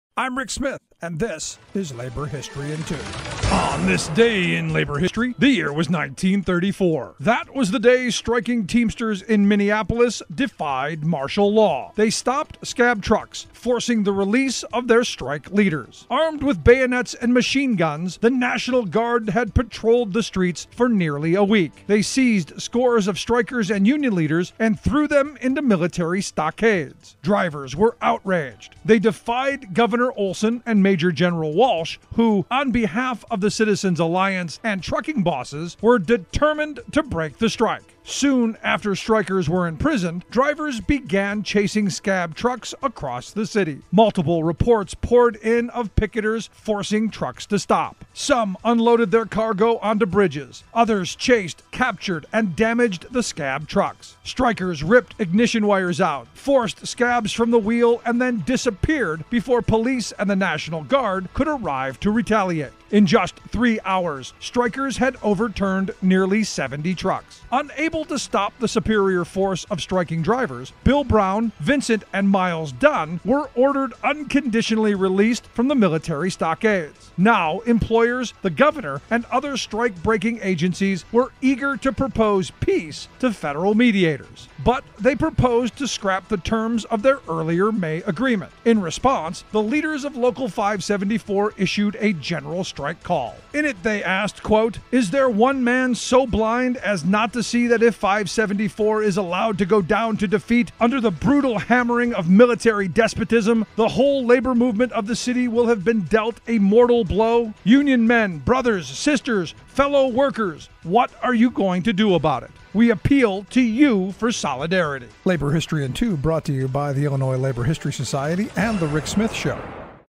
August 2 - Teamsters Defy Martial Law
On this day in labor history, the year was 1934.

That was the day striking Teamsters in Minneapolis defied martial law.

They stopped scab trucks, forcing the release of their strike leaders.

Armed with bayonets and machine guns, the National Guard had patrolled the streets for nearly a week.

They seized scores of strikers and union leaders and threw them into military stockades.

Drivers were outraged.

They defied Governor Olson and Major General Walsh who, on behalf of the Citizens Alliance and trucking bosses, were determined to break the strike.

Soon after strikers were imprisoned, drivers began chasing scab trucks across the city.

Multiple reports poured in of pickets forcing trucks to stop.

Some unloaded their cargo onto bridges.

Others chased, captured and damaged trucks.

Strikers ripped ignition wiring out, forced scabs from the wheel and then disappeared before police and the National Guard could arrive to retaliate.

In just three hours, strikers had overturned nearly 70 trucks.

Unable to stop the superior force of striking drivers, Bill Brown, Vincent and Miles Dunne were ordered unconditionally released from the military stockades.

Now employers, the governor, and other strike breaking agencies were eager to propose peace to federal mediators.

But they proposed to scrap the terms of their early May agreement.

In response, the leaders of Local 574 issued a General Strike call.

In it they asked, “Is there one man so blind as not to see that if 574 is allowed to go down to defeat, under the brutal hammering of military despotism, the whole labor movement of the city will have been dealt a mortal blow?

Union men, brothers, sisters, fellow workers!

What are you going to do about it?

We appeal to you for solidarity!”